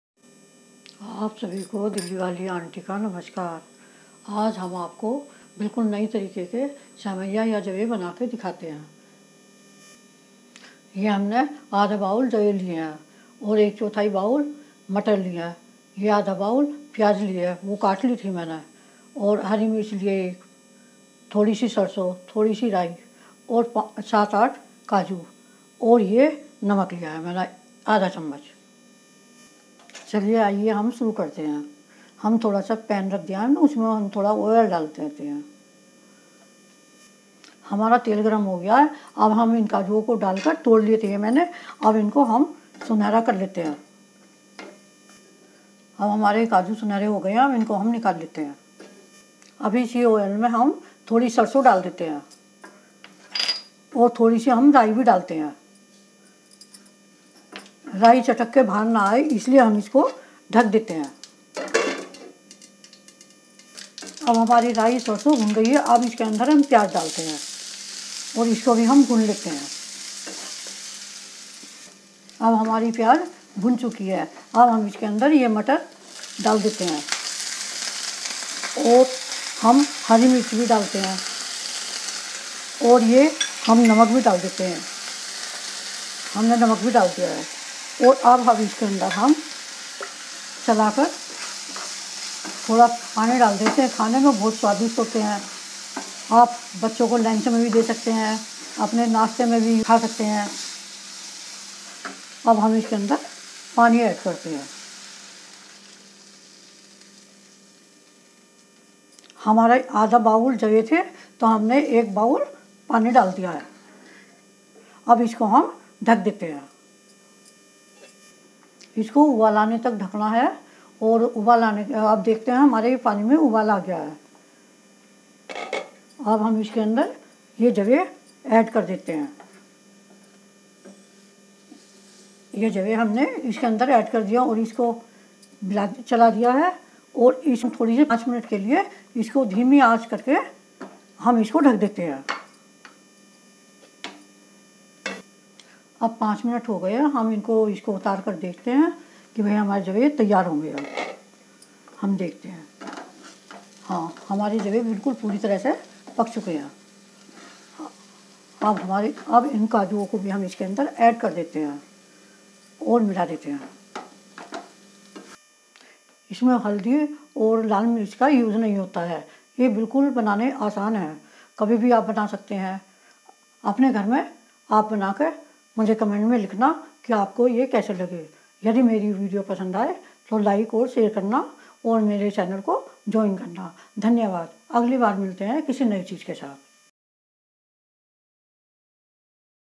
Vermicelli Pulav Recipe - Semiya Pulao Recipe -Vermicelli Upma | Breakfast recipes एक बार जरूर बनाये
A popular South Indian breakfast dish vermicelli upma is also known as semiya upma. It is a quick and healthy snack that you can cook easily in just 20 minutes with simple ingredients. A light and healthy breakfast option, this upma recipe is made from vermicelli, a traditional type of pasta round in section similar to spaghetti. A delicious and quick snacking option for all your hunger pangs. This is so light that it is known to be an all-time meal and not just restricted to breakfast or brunch.